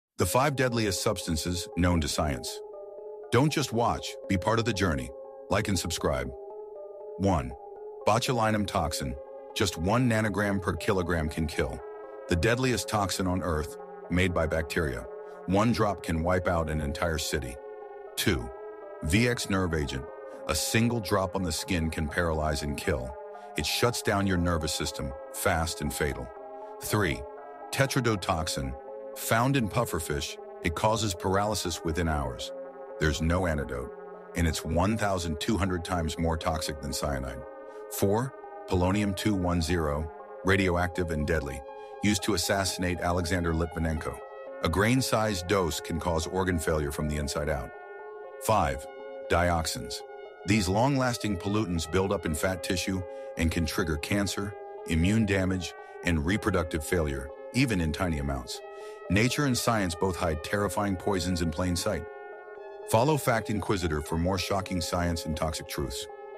The 5 Deadliest Substances Known to Science!
These are not your everyday poisons. From microscopic toxins to radioactive assassins, these five substances are among the deadliest ever discovered. Learn how they kill, where they’re found, and why even the tiniest amount could mean instant death. You’ll never look at science the same way again...

🔬 Follow Fact Inquisitor for more terrifying truths from the world of science and history.